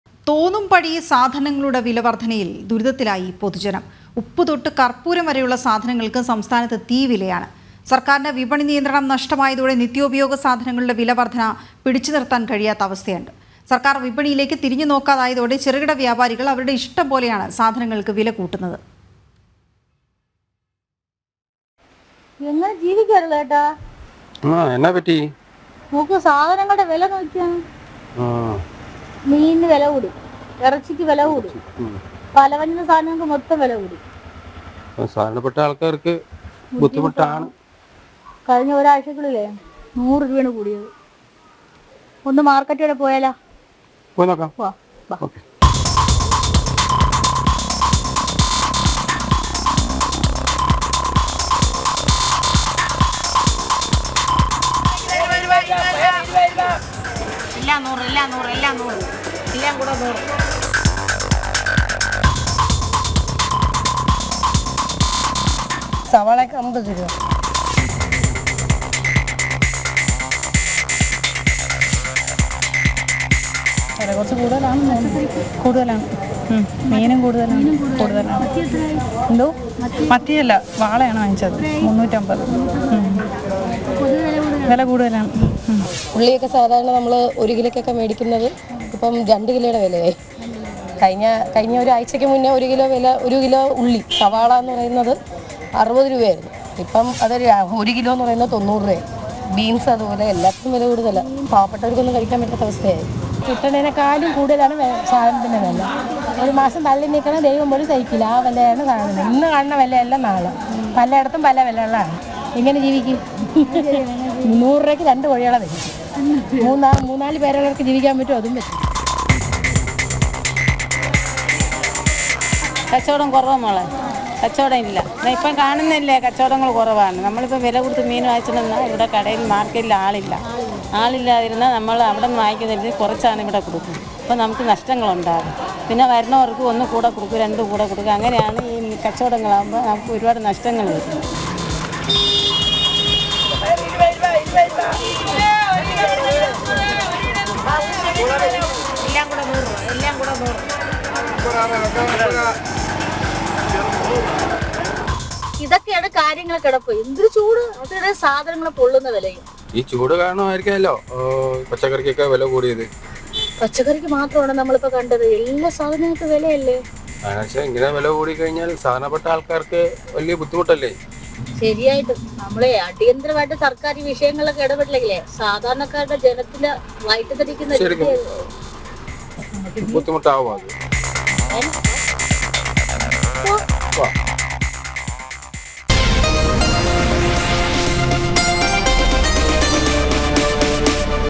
ഉപ്പുതൊട്ട് കർപ്പൂരം വരെയുള്ള സാധനങ്ങൾക്ക് തീവില | INFLATION
തോന്നുംപടി സാധനങ്ങളുടെ വിലവർധനയിൽ ദുരിതത്തിലായി പൊതുജനം. ഉപ്പുതൊട്ട് കർപ്പൂരം വരെയുള്ള സാധനങ്ങൾക്ക് സംസ്ഥാനത്ത് തീ വില. സർക്കാരിന് വിപണി നിയന്ത്രണം നഷ്ടമായതോടെ നിത്യോപയോഗ സാധനങ്ങളുടെ വിലവർധനവ് പിടിച്ചുനിർത്താൻ കഴിയാത്ത അവസ്ഥ. സർക്കാർ വിപണിയിലേക്ക് തിരിഞ്ഞു നോക്കാതായതോടെ ചെറുകിട വ്യാപാരികൾ അവരുടെ ഇഷ്ടം പോലെയാണ് സാധനങ്ങൾക്ക് വില കൂട്ടുന്നത്.